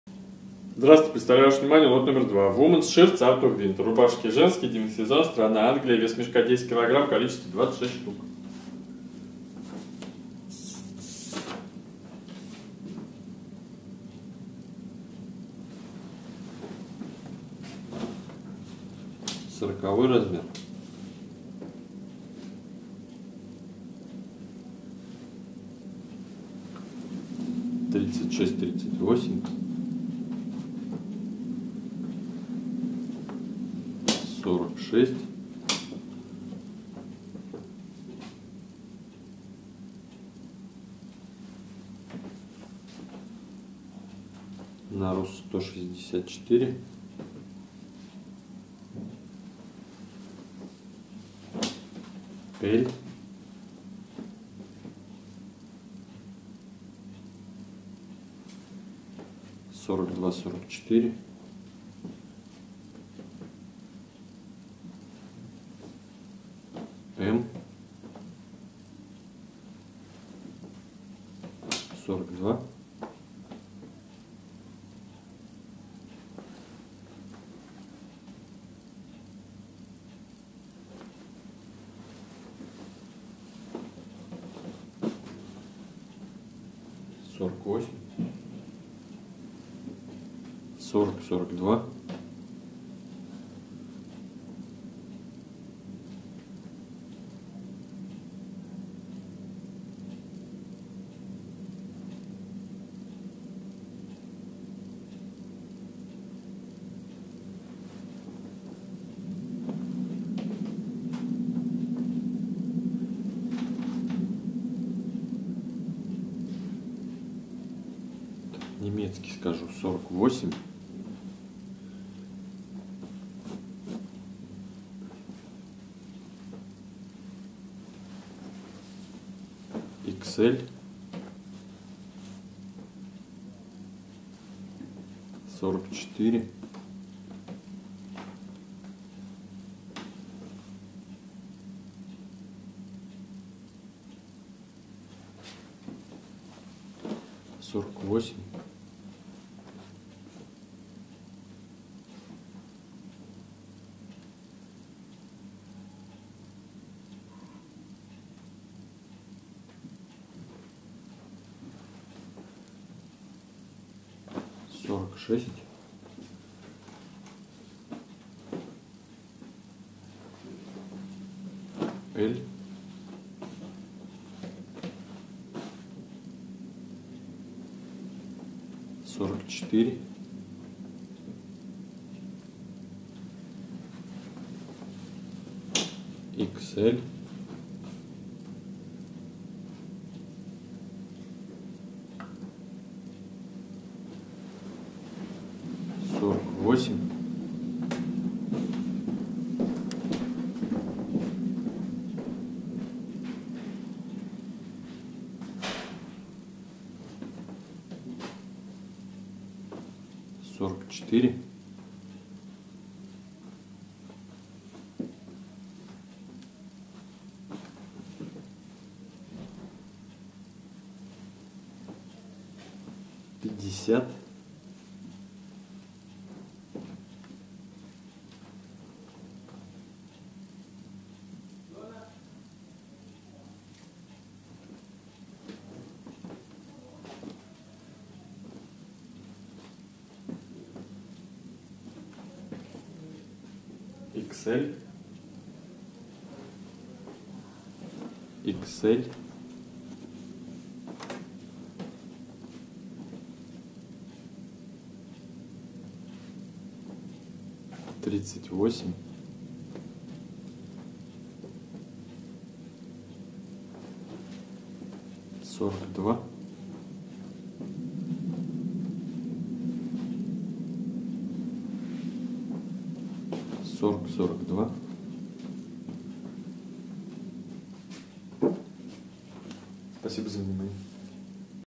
WOMENS SHIRTS Autumn Winter 2,Цена за кг.(по уценке): 2 евро, секонд хенд одежда оптом
WOMENS SHIRTS Autumn-Winter (Лот №  2 )
Женские рубашки (секонд хенд)
Демисезон
Страна: Англия
Цена за кг.(по уценке): 2 евро
Вес мешка,кг.: 10
Цена за мешок: 20 евро
Кол-во в мешке: ~ 26